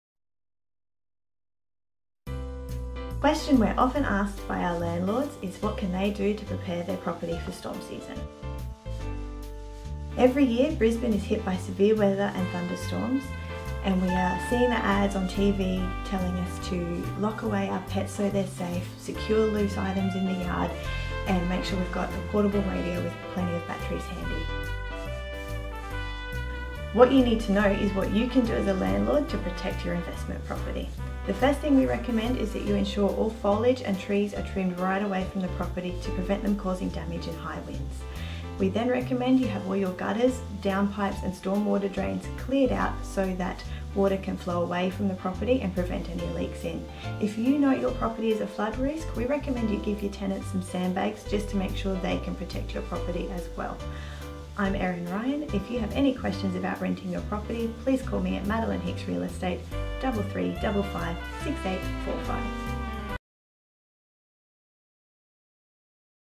Ask Madeleine - "What can I do to prepare my property for storm season? Property Management
What can I do to prepare my property for storm season? Every year we are hit with severe thunderstorms. Watch as Erin explains what you can do as a landlord to protect your property.

**LINDA insert property description here**